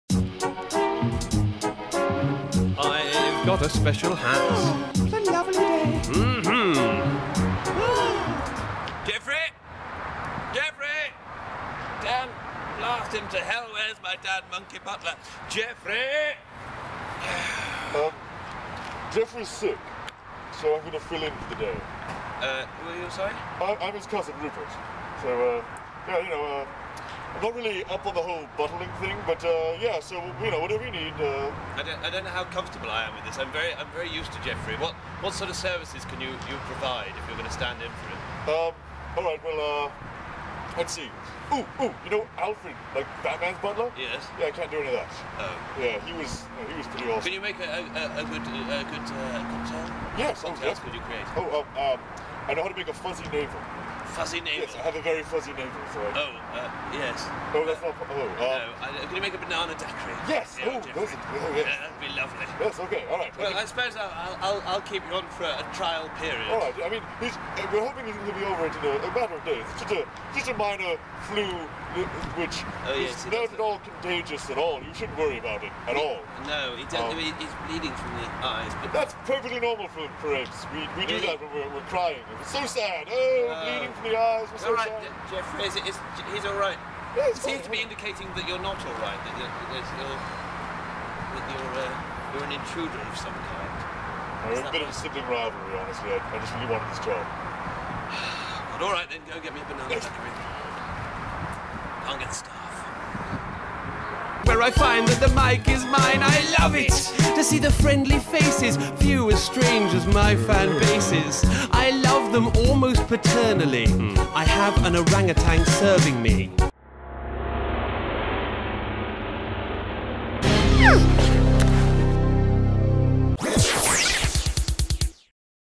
Rupert Meets Professor Elemental
Professor Elemental is used to a specific lifestyle, catered to thanks to his "monkey butler" Geoffry, But today at Elemental Manor, Geoffry has taken ill, and its up to his cousin to step in.

Filmed on location at Professor Elemental's live show in May of 2011 Dogtoon Media would like to thank Professor Elemental for goofing around with us.